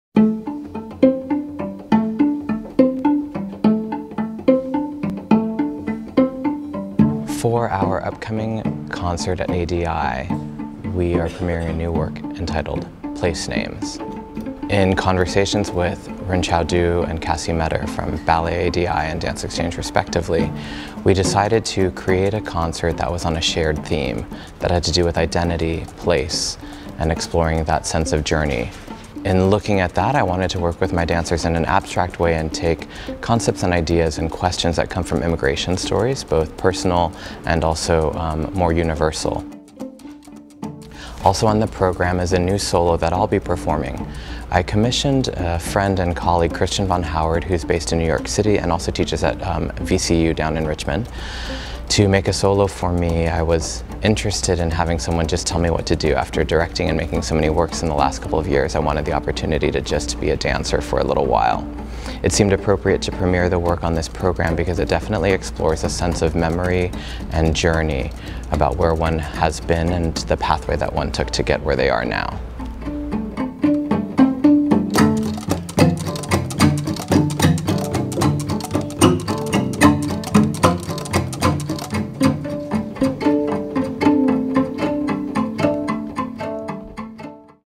Behind Closed Doors: Place Names
A Behind Closed Doors look at CKM&A's new work

Place Names

Premiering in
IMPRINTS FROM THERE TO HERE
Ballet ADI / Christopher K. Morgan & Artists / Dance Exchange
June 15 at 7:30p & June 16 at 2:00p

Three DC companies examine notions of self and place through movement.

Place Names
A new work by Christopher K. Morgan, Place Names explores how names, language and labels can shape one's identity.  The places one leaves behind, runs toward and arrives at can change the perception of that person and how they are perceived by others. The piece asks, "How does one integrate all of these into a cohesive self?"

Also on the program, Morgan will present the world premiere of a currently untitled solo by renowned Choreographer Christian Von Howard.  Created on Morgan with the support of an Individual Artist Award from the Arts and Humanities Council of Montgomery County, the work features original music by Kevin Keller and text by Pablo Neruda.